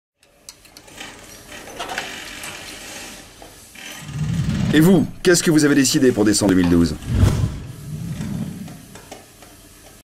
TPE garage
En décembre 2012, 4 millions de salariés des TPE, Très Petites Entreprises, auront leur élection. Pour la première fois, ces salariés d'entreprises de moins de 10 salariés pourront voter pour désigner leur syndicat. En effet, ces entreprises (TPE) ne sont pas dotées d'instances élues du personnel (délégué du personnel -- à partir de 11 salariés, comité d'entreprise -- pour les plus de 50 salariés) et la représentation syndicale y est donc la plupart du temps absente.Certes, dans un premier temps, ces élections, qui seront organisées sur le plan national, n'apporteront pas de droits nouveaux, leur objectif étant d'évaluer la représentativité des syndicats dans les TPE.Mais avec ces élections, il faut faire sortir de l'ombre, du point de vue de leurs droits, les salariés des TPE, notamment de l'artisanat, des professions libérales, du commerce de proximité (alimentaire et non alimentaire), des services dans de nombreux secteurs (y compris des services à la personne).